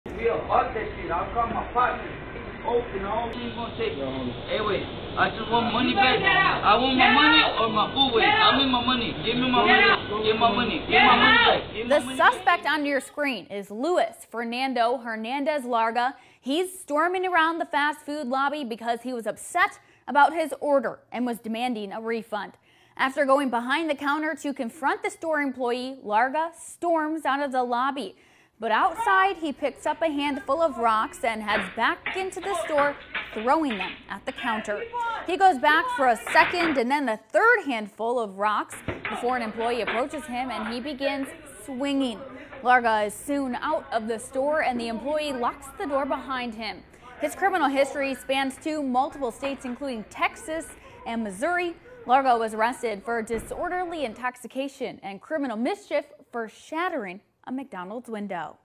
Florida man caught on camera throwing rocks inside McDonald's in Lee County
Wild video shows a Florida man throwing rocks inside a McDonald’s in Lee County.